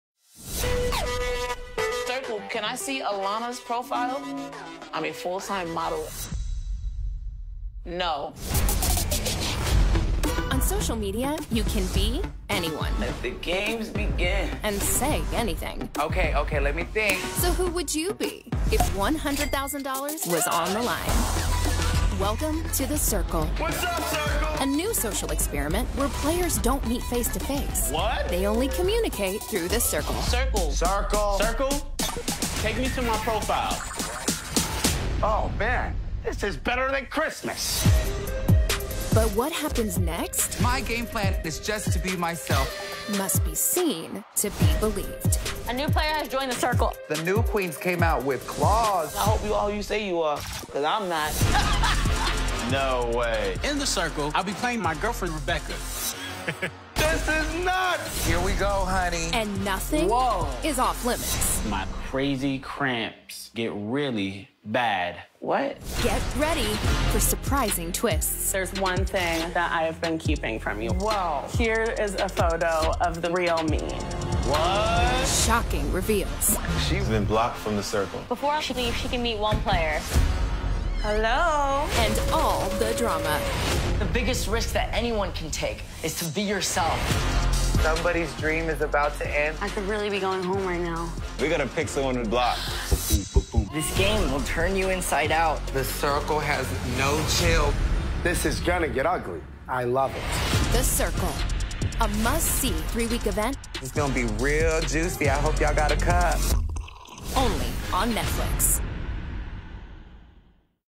Chris Sapphire- I'm On The Circle NETFLIX USA!
I'm so excited to finally be able to announce that I am going to be on @TheCircleNetflix,  a new reality-competition series where players can be whoever it takes to win.  TheCircle, a Netflix three-week event, starts Jan 1. ⭕️